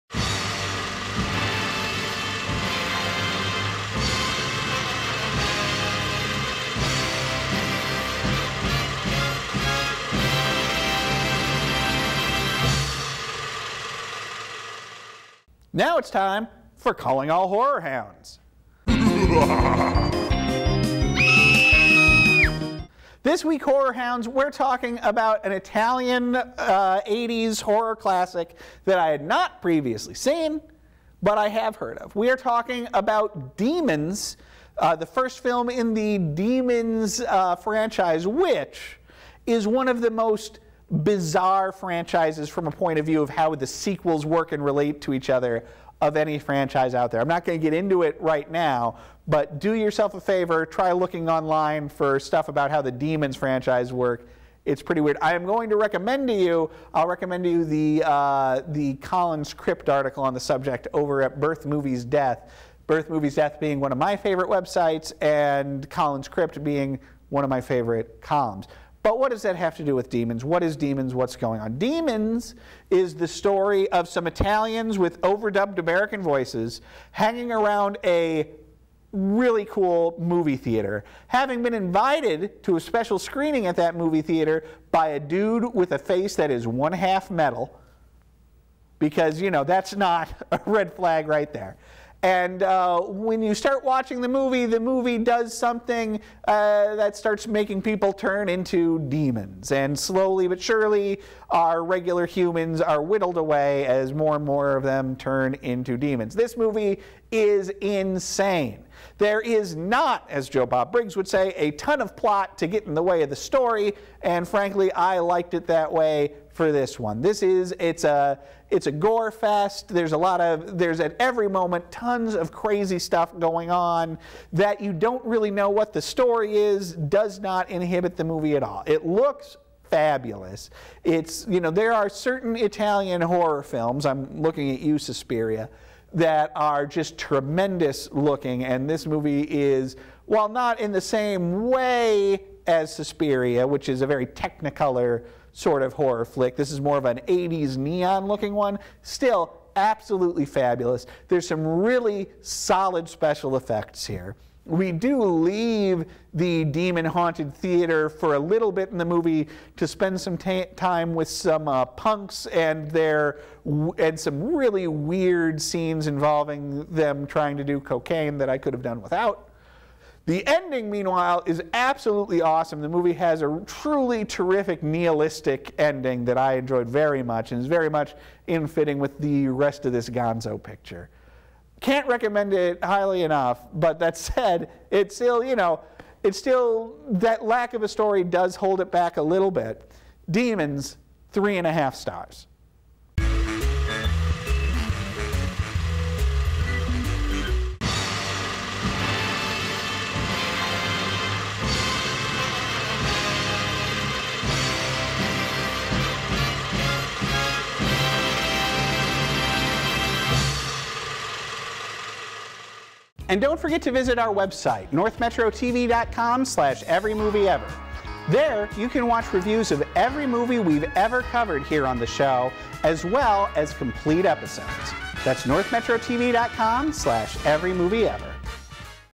Every Movie Ever - Demons
We're reviewing every movie ever made. Today, Demons, the superior Italian gore fest.